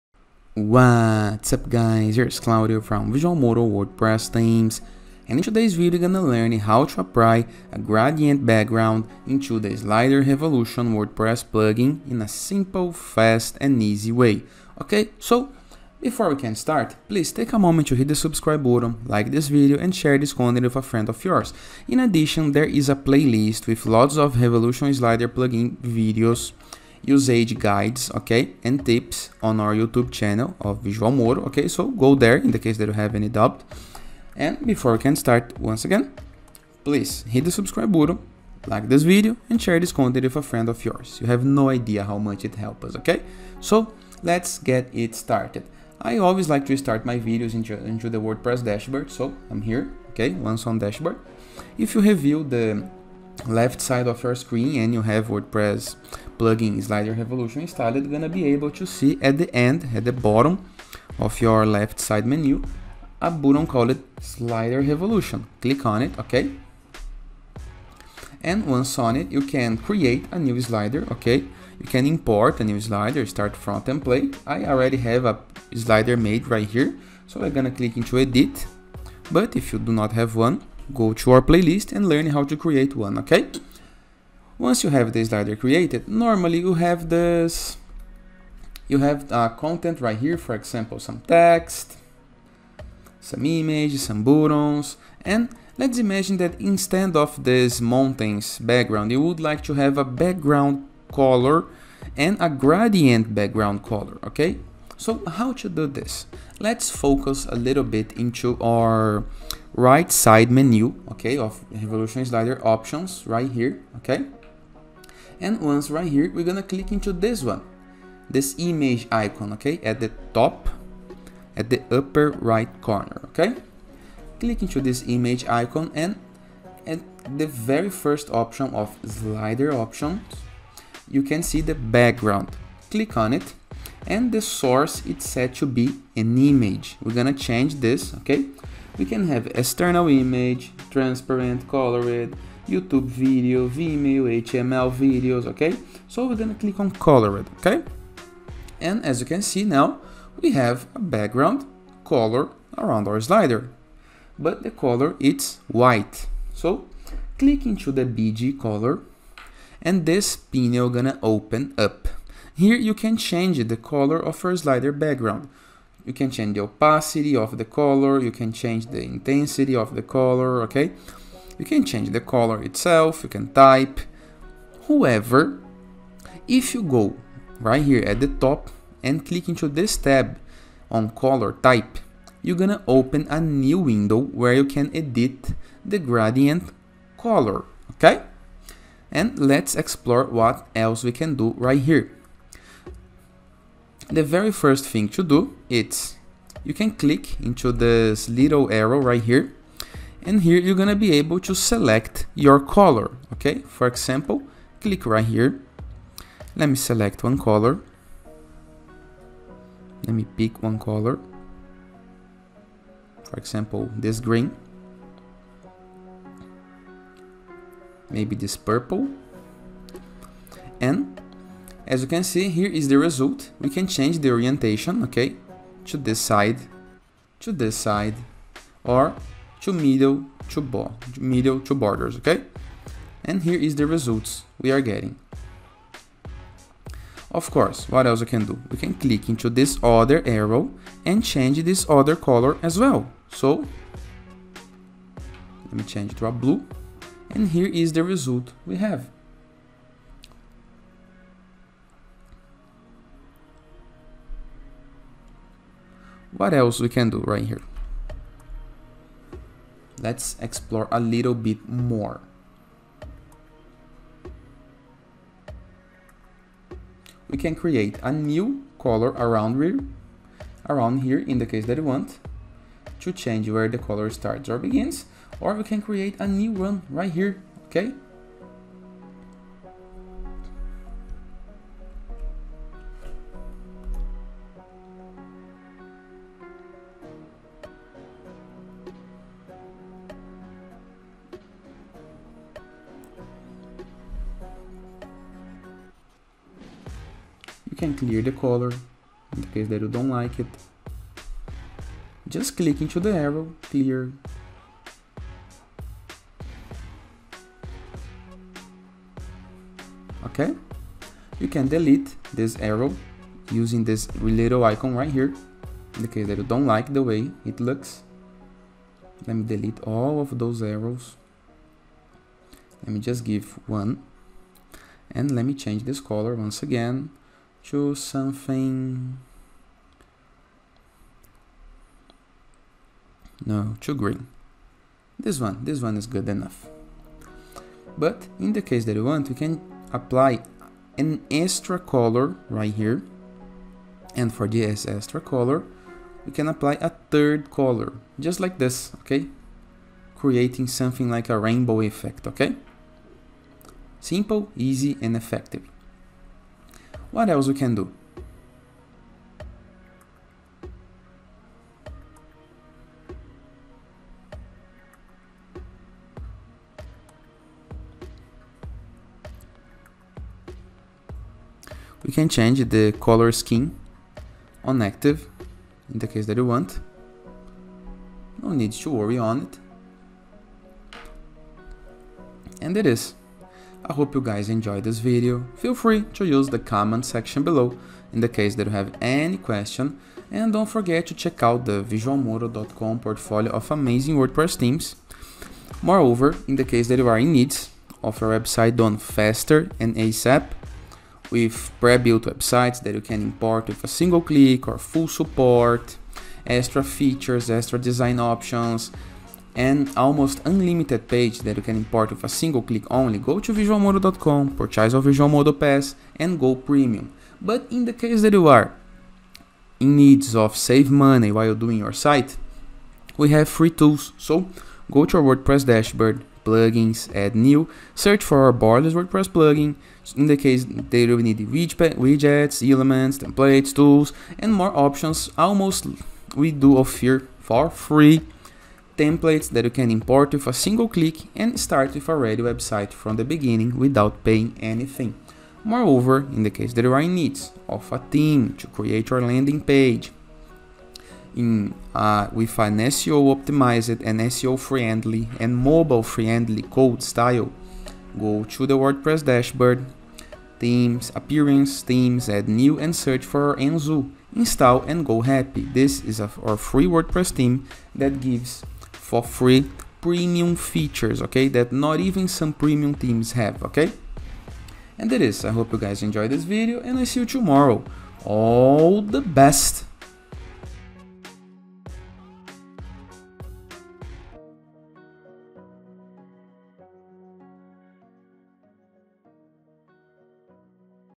Gradient Background Colors Usage Guide on Revolution Slider WordPress Plugin
In today's video tutorial, we'll learn a simple, fast and effective way to use gradient background colors in the revolution slider WordPress plugin

Tutorial on How to Create Unique Gradient Background Themes Learn how to take a new and unique look at the gradient background trend with Slider Revolution and pre-made design templates. These days, as people spend more and more time online, it takes a very specific experience to make them stop and focus more on one website than another.

Gradients have been very popular over the past few years, so you'll see them a lot on websites - as backgrounds, buttons, fonts, and more. However, that doesn't mean gradients aren't a web design trend worth developing. All you have to do is create a unique trend.

Slider Revolution makes it easy. In the following tutorial, we will show you how to change the background effect hero template to create your own fantastic gradient background design.

When you draw gradient backgrounds, you have to think about what kind of response you want to get.

Lighter, softer gradients and less color contrast create a more comfortable and relaxing environment. On the other hand, a color gradient with more intense colors or extreme color contrast ensures a stronger response.

If you need help creating a gradient scheme, start with a tool like Coolors. Click the Browse Gradients tab and explore endless examples of gradients: To edit a slideshow's background, open the slideshow options in the editor. Go to "Background" and "Sources". You can add images and videos, get content from other sources, and apply colors to your background. To create a gradient, keep Color selected and click the BG color box to open the editor:

There are two settings here: one for solid backgrounds and one for gradients. Gradients are currently configured. We wanted to stay put and work on color.

To change the color, click the pointer at the bottom of the gradient bar. In the Color field, enter the appropriate hex code. If you want to use fewer colors, click the pointer you want to delete, then click the trash can (delete) icon just below the bar. To add color again, just click on the gradient bar and a new pointer will be created for you.